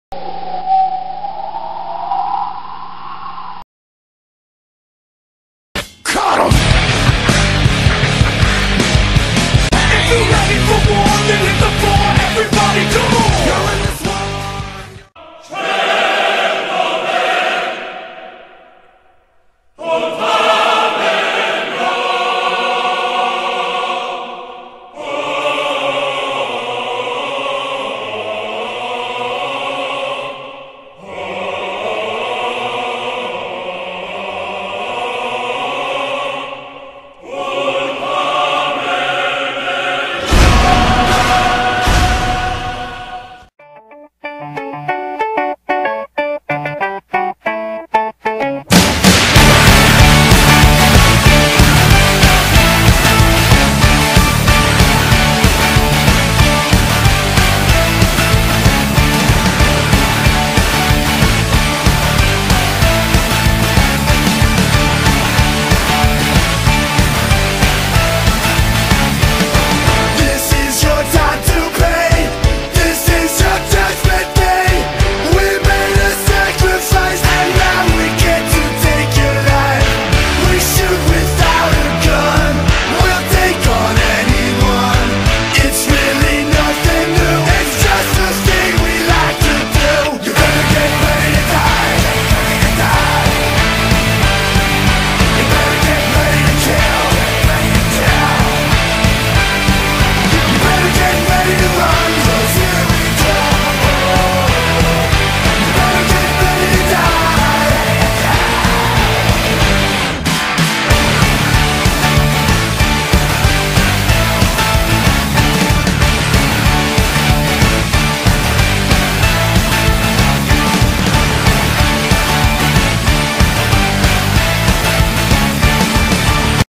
MoC update: Makuta Zerone
not a MAJOR update but a small one plus i needed an excuse to upload video of him XD now for those of you who dont know who zerone is he was originaly my self moc till i made h2o and i made zerone my 2nd self MoC so enjoy people :)